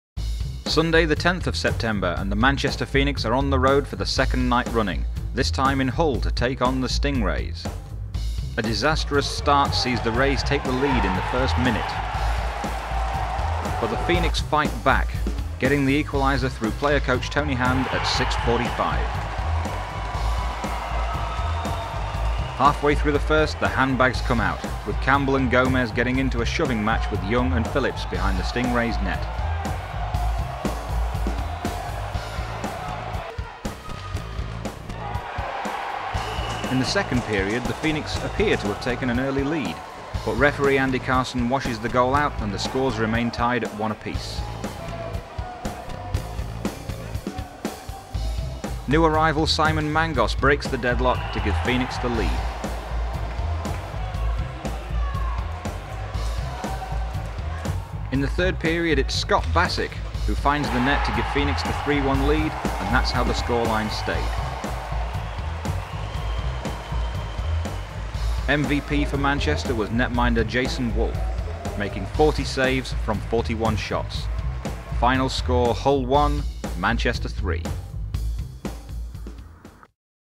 [archive] 10th Sep 2006 - Stingrays Away game highlights
Game highlights from the Hull Stingrays vs Manchester Phoenix game held on 10th September 2006.